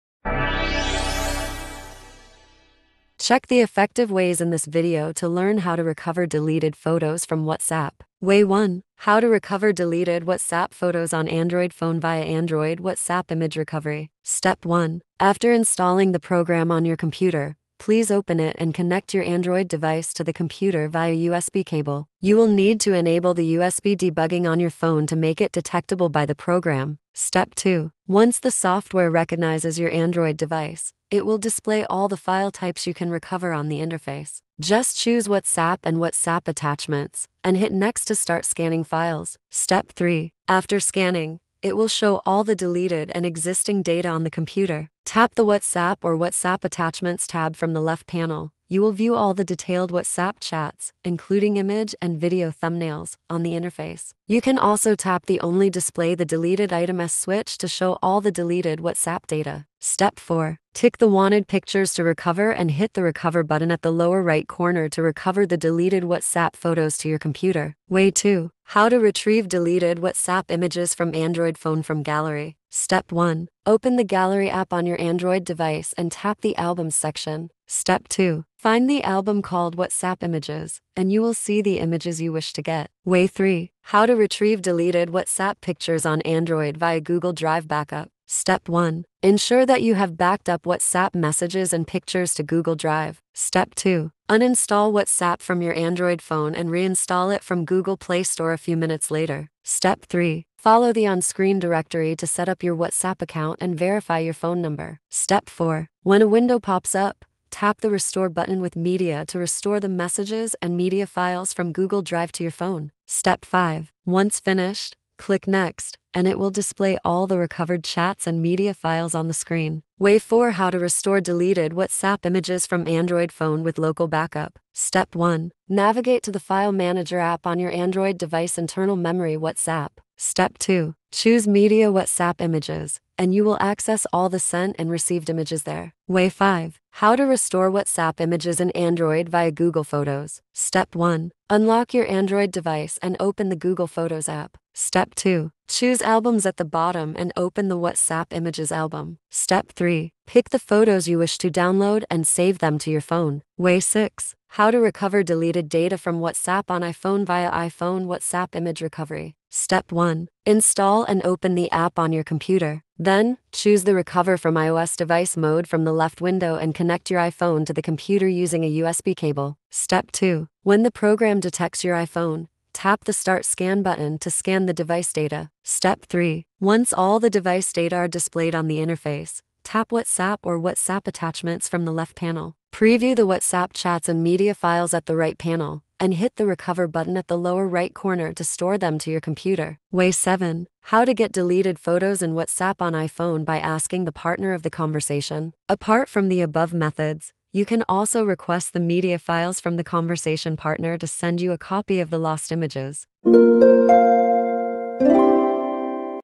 How to Recover Deleted Photos in WhatsApp on Android and iPhone with Ease
Desperate for a way to recover the deleted WhatsApp photos? We show you how to recover deleted photos from WhatsApp on Android and iPhone in 7 trouble-free methods in this video.